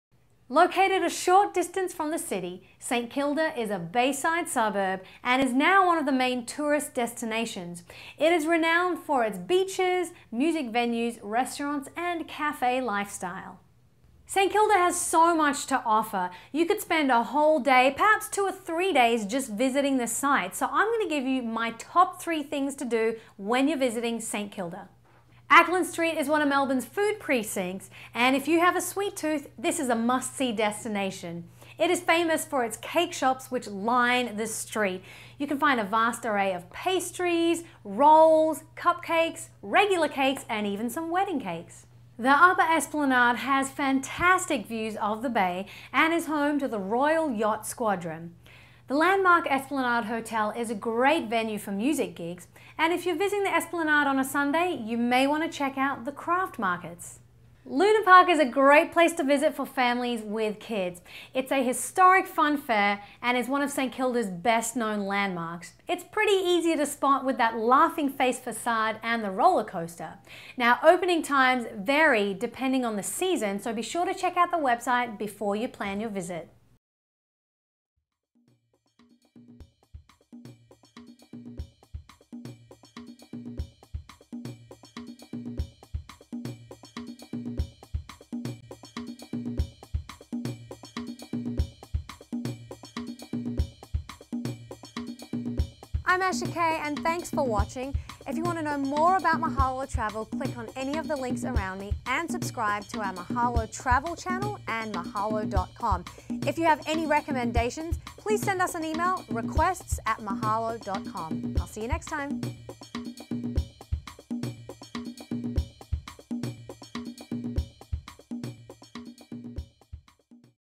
St. Kilda - Melbourne, Australia
This historic Melbourne destination is home to one of Melbourne's premiere food precincts, Acland Street, best known for its swanky cafes and cake shops. Asha shares a few quick facts about this beautiful location to help you plan your visit the next time you are in Melbourne, Australia.